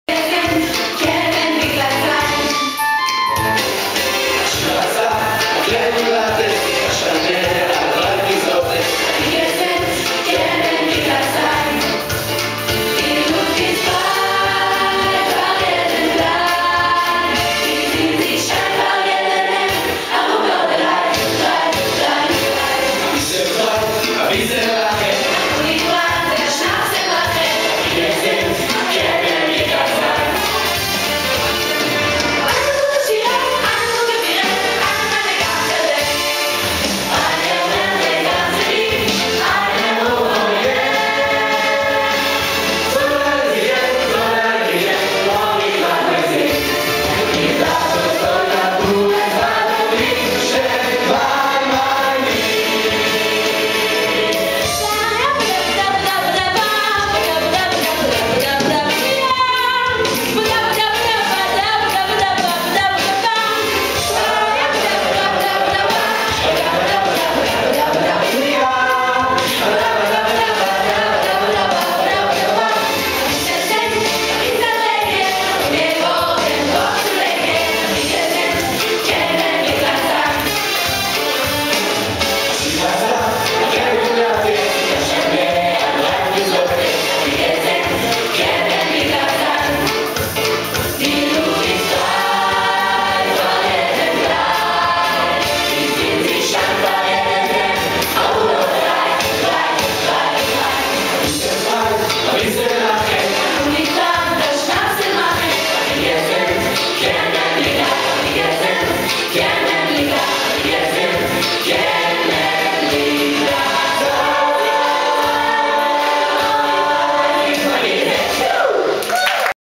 Israeli Scout Friendship Caravan Performance
Israeli Scout Friendship Caravan visits Memphis Jewish Home & Rehab.